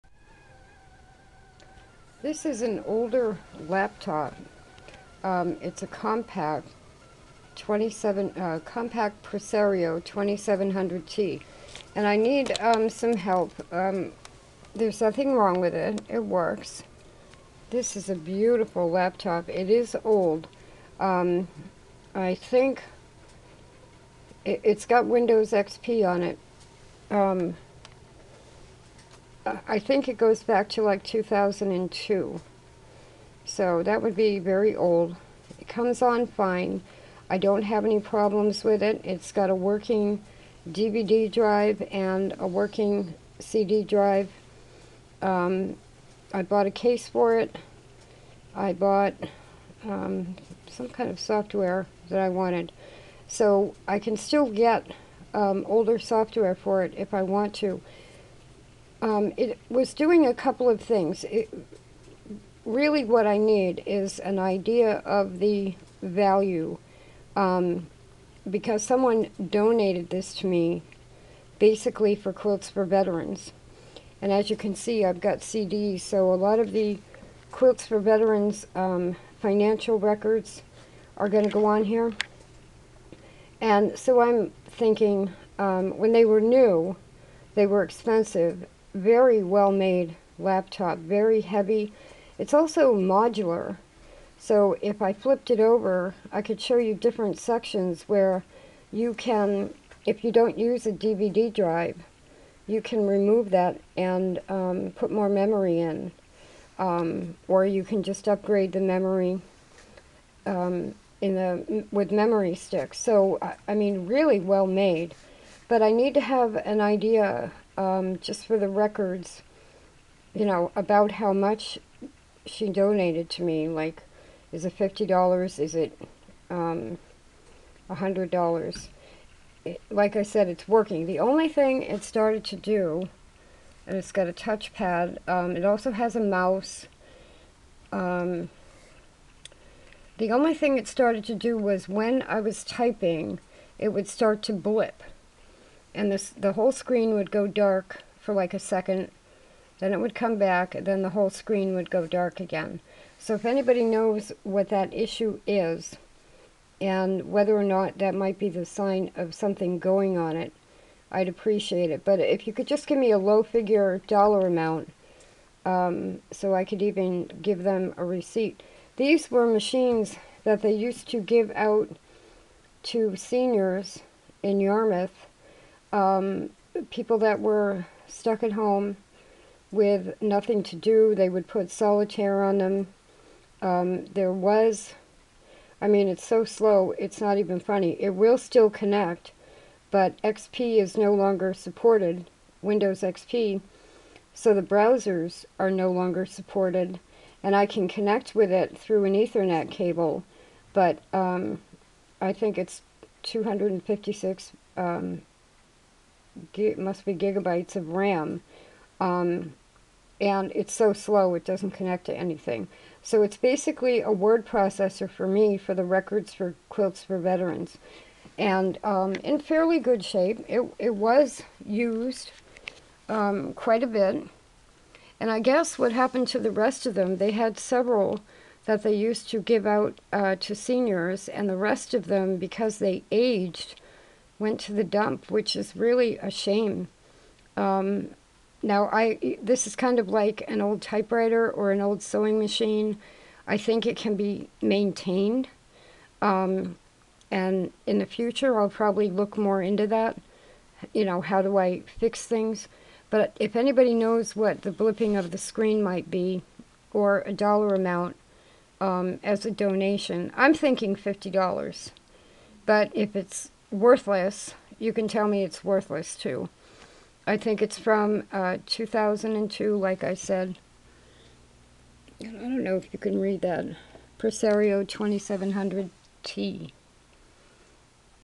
Compaq Presario 2700T Laptop Help Please
An older laptop given to me for use for Quilts For Veterans. The screen blip has only happened twice.  Any suggestions are welcomed.